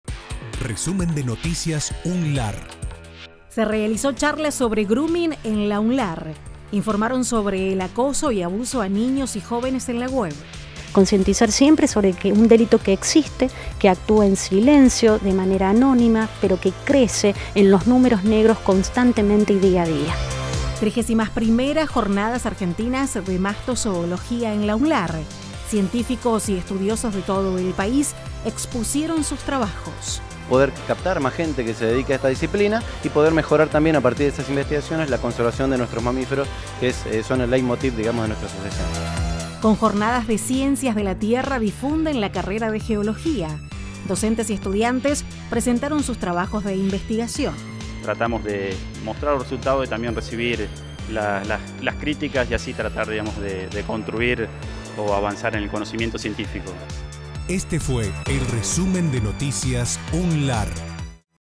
Resumen de Noticias UNLaR 25-10-18
- Charla sobre Grooming;
- 31° Jornadas Argentinas de Mastozoología;
- 4° Jornada de la Tierra